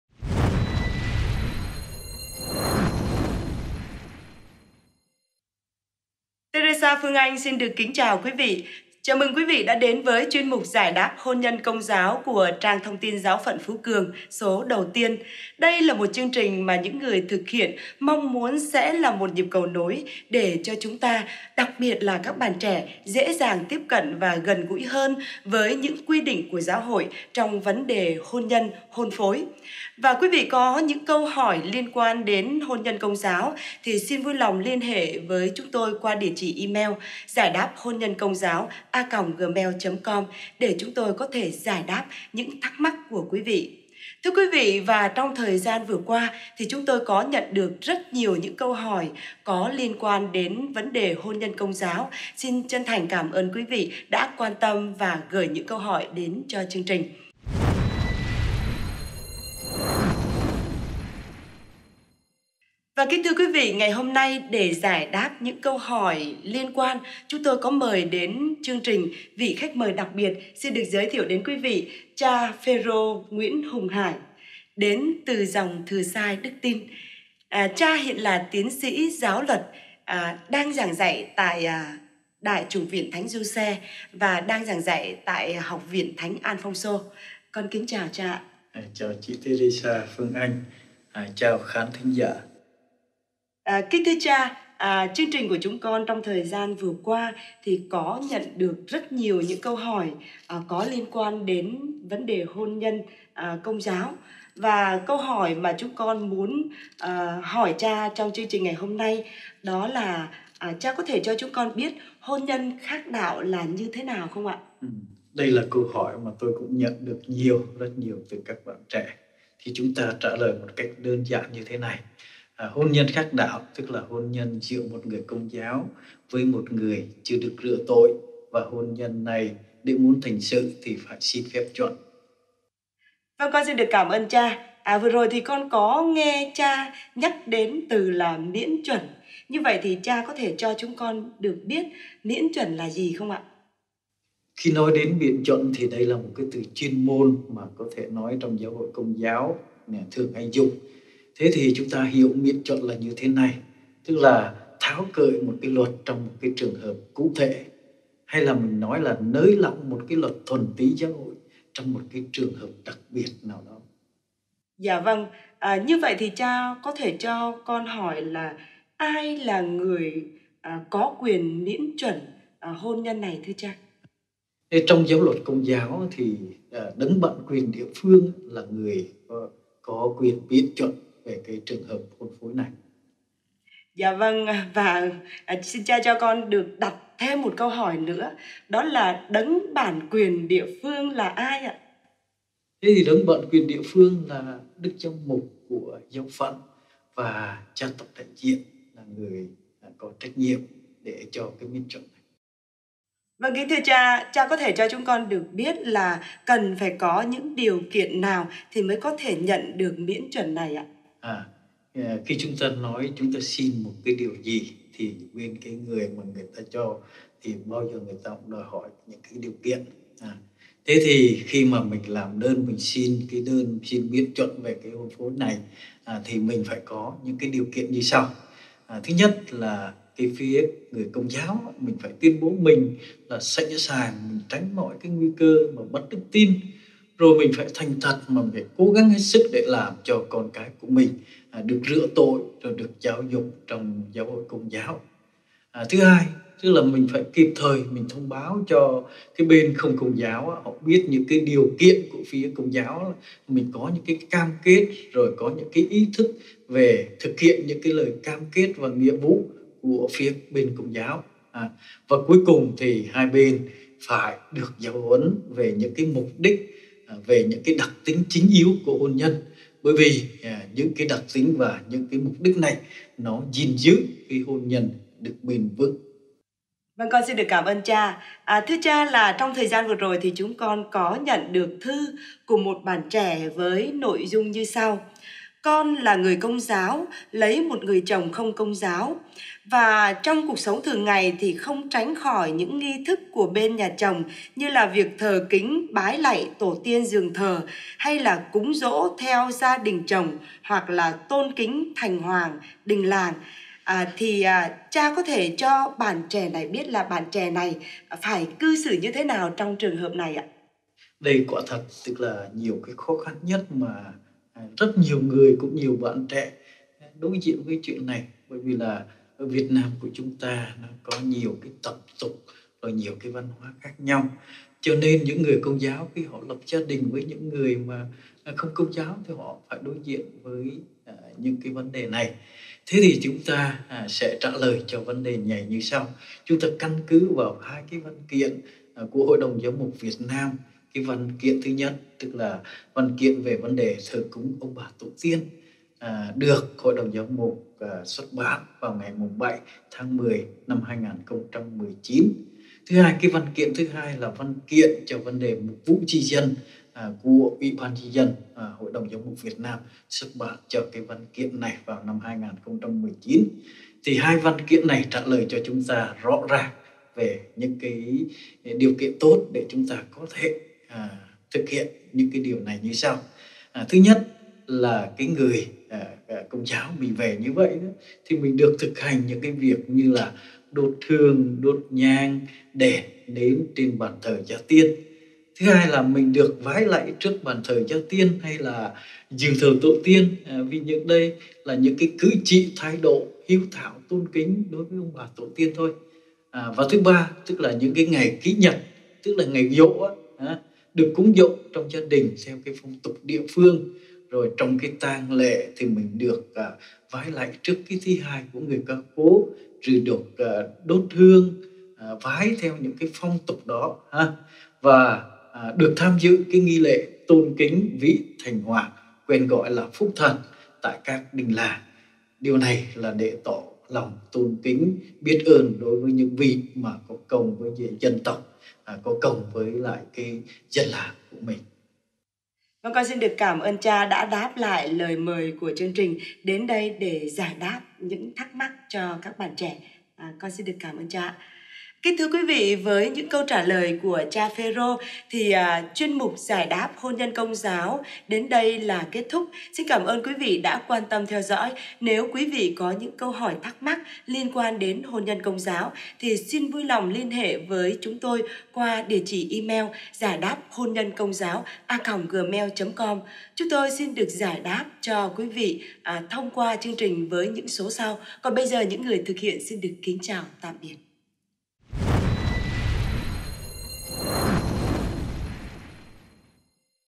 HÔN NHÂN CÔNG GIÁO – Hôn Nhân Dị Giáo P1
GIẢI ĐÁP HÔN NHÂN CÔNG GIÁO – Hôn Nhân Dị Giáo P1
Lm. Phêrô Nguyễn Hùng Hải, MF.
1. Hôn nhân khác đạo
2. Hôn nhân dị giáo
3. Miễn chuẩn
4. Bản quyền địa phương
5. Chuẩn khác đạo
6. Phép Chuẩn
7. Thờ cúng tổ tiên
8. Hôn nhân công giáo
9. Giải đáp hôn nhân công giáo